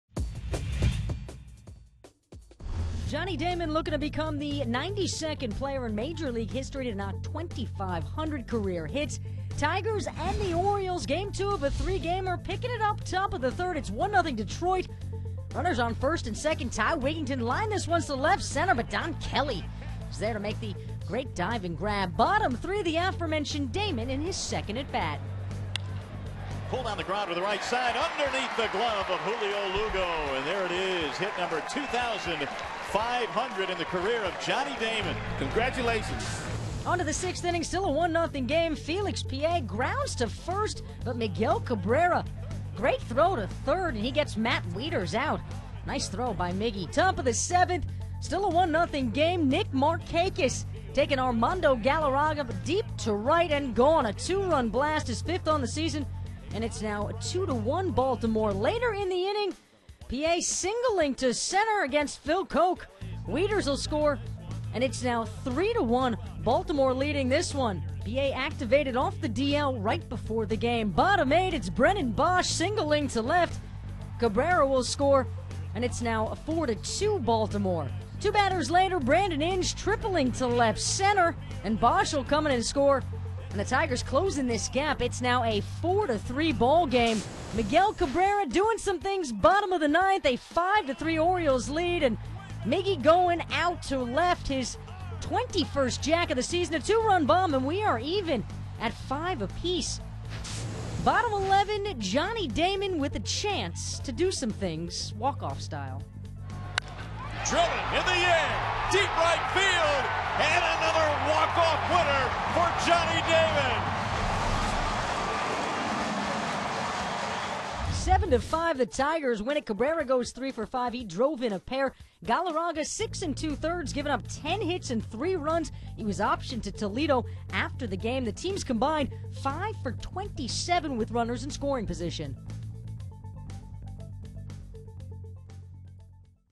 2010/07/06 Recap: BAL 5, DET 7 F/11
7/6/10: Damon achieves milestone in walk-off win